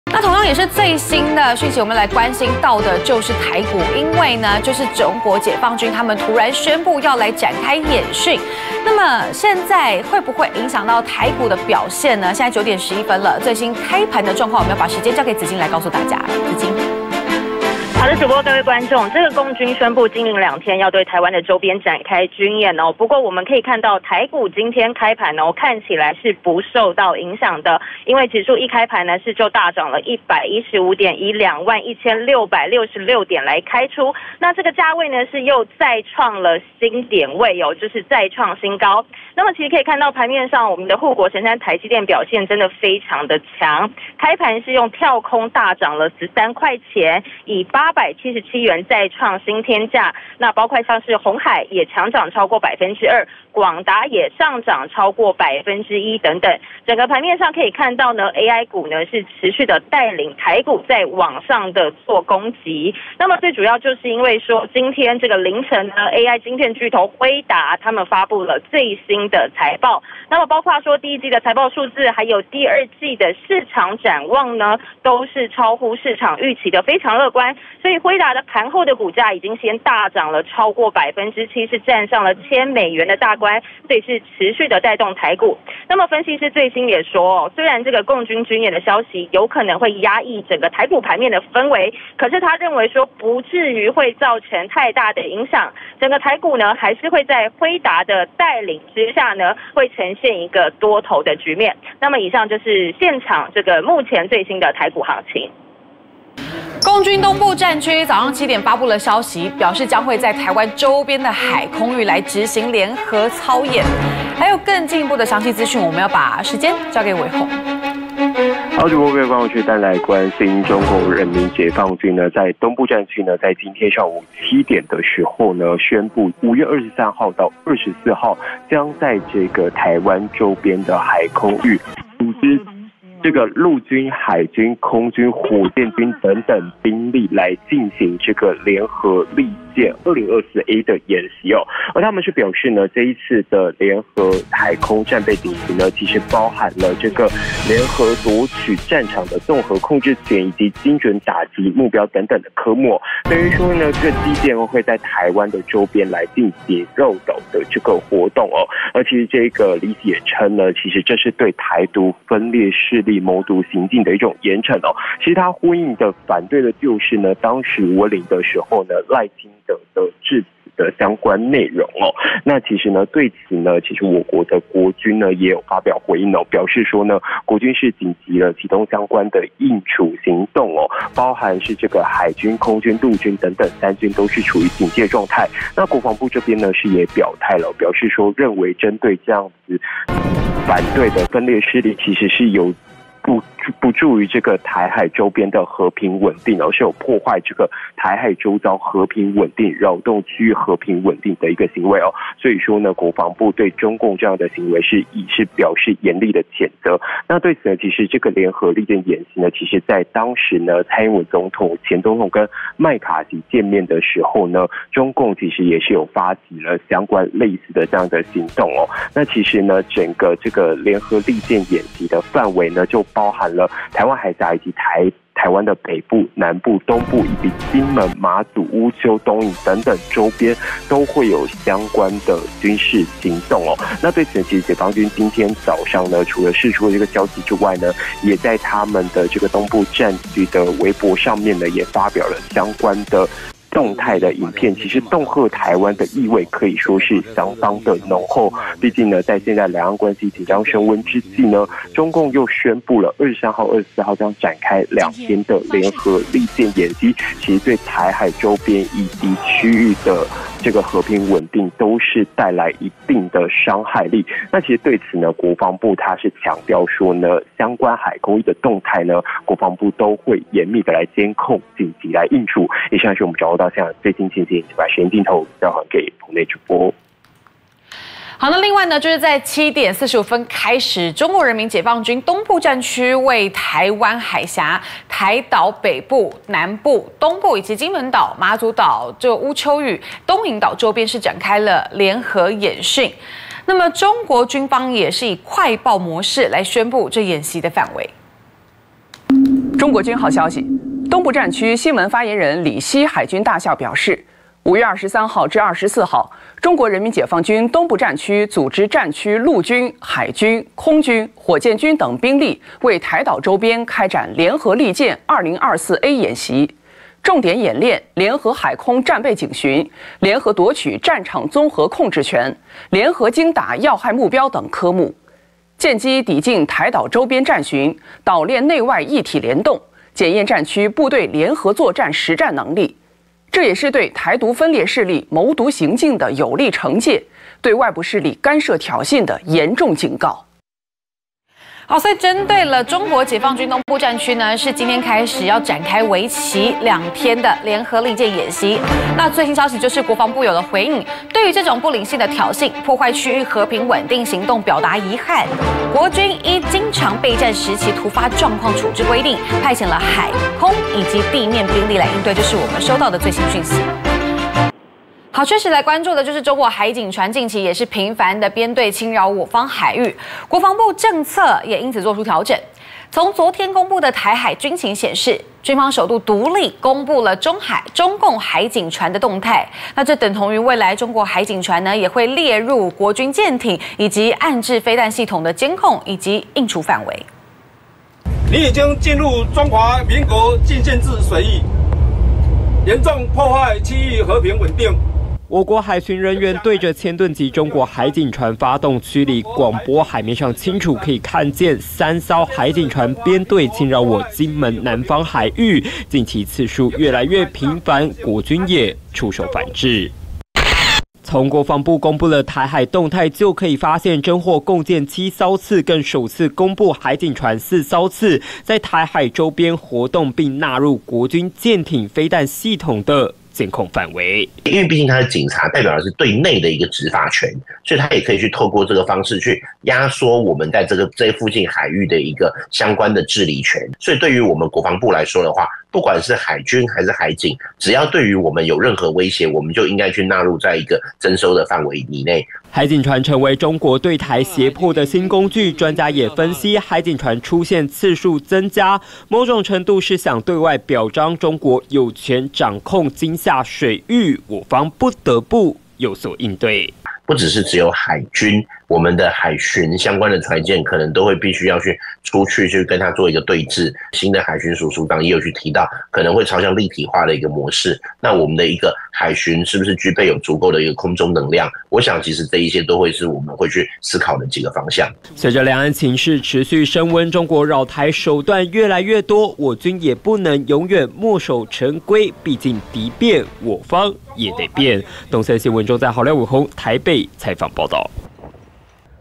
無懼中共軍演 ! 台股開盤仍大漲百點
台灣北、東、南納範圍 解放軍突襲「圍台軍演」
共軍宣布台灣周邊軍演 國軍緊急應處：破壞區域穩定
共軍突宣布軍演！台股無懼開盤仍大漲百點
中共武嚇「嚴防灰色衝突」國防部台海軍情「首曝海警船」